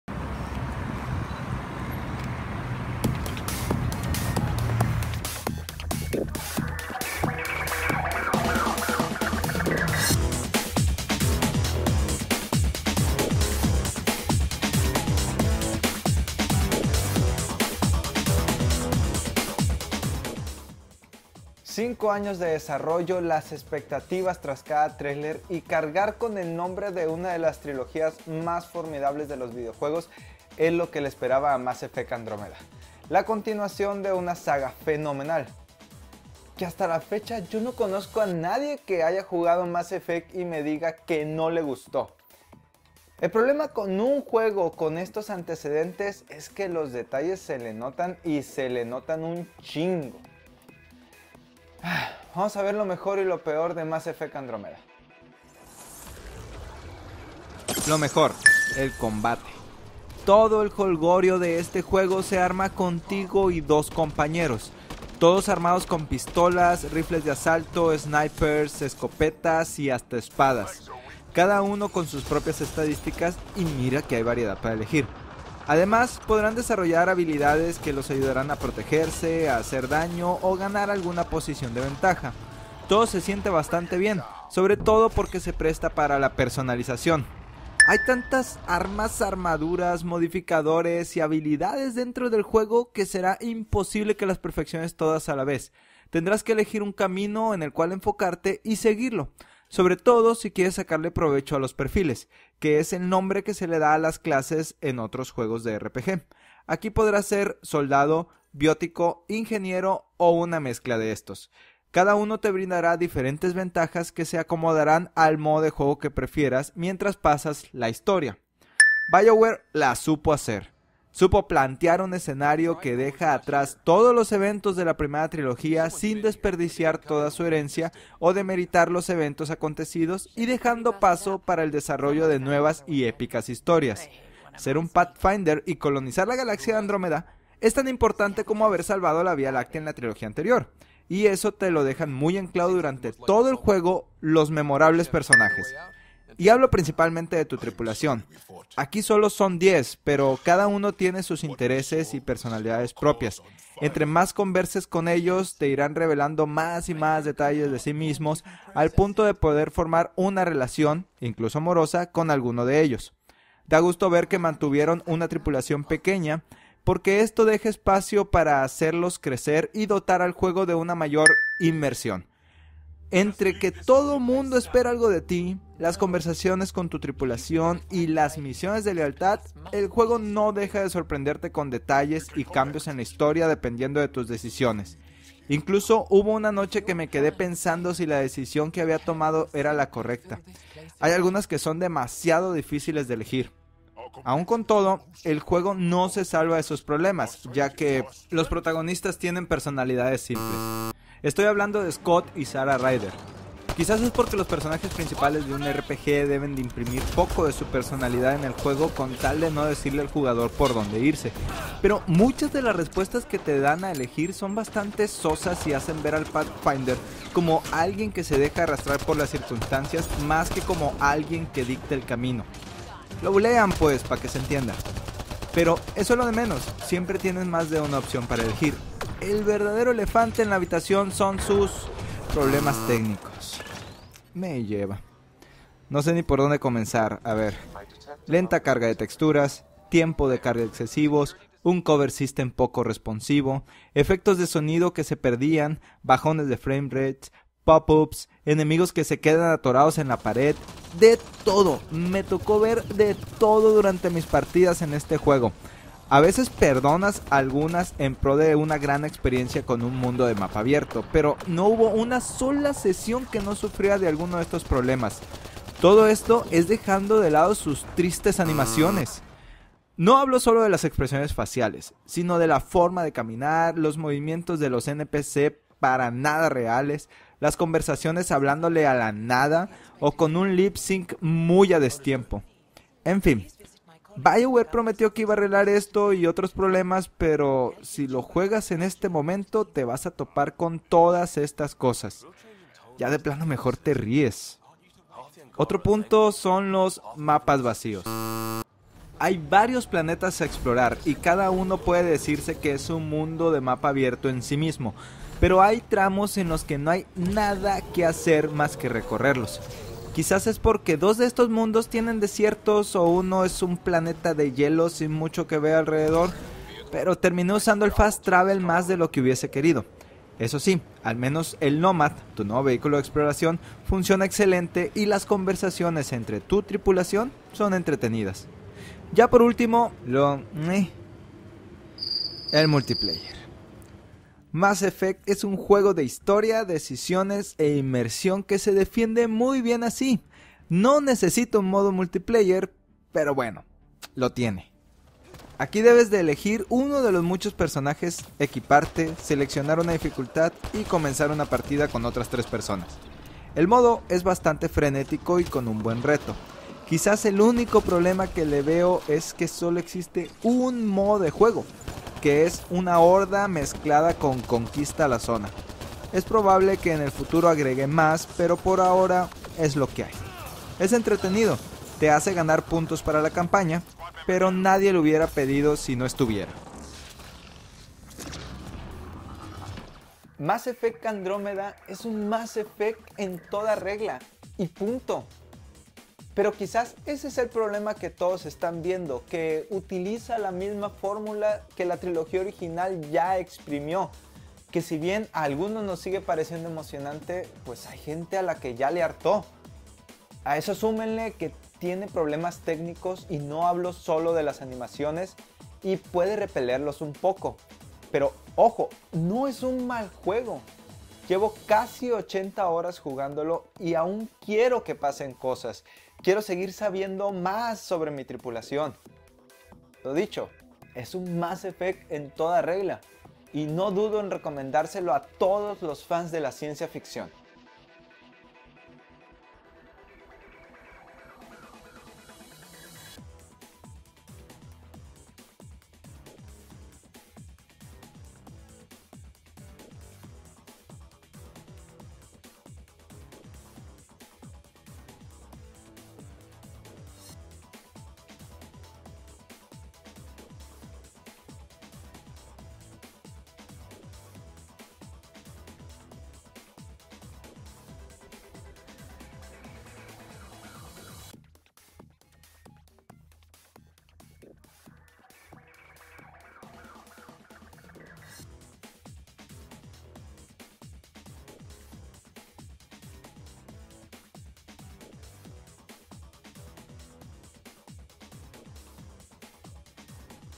Lo Mejor y Lo Peor: Mass Effect Andromeda
Desarrollado por: Bioware, Electronic Arts
Publicado: 21/marzo/2017
Plataforma: PS4, Xbox ONE, PC
El juego fue proporcionado por Electronic Arts para propósitos de reseña